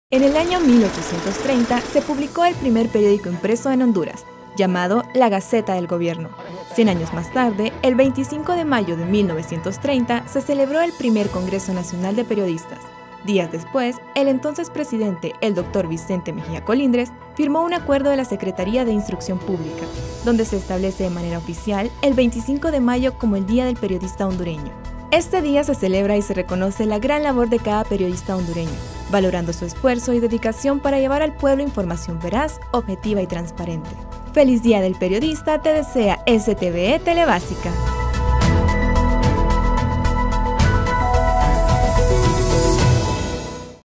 Día del Periodista
Este 25 de mayo, le deseamos un feliz Día del Periodista a todos los que cumplen con informar al pueblo. ¡Feliz día periodistas!

"Transformando la nación con mejor educación". Recuerda ¡La educación cambia una nación! Secretaría de Educación Honduras.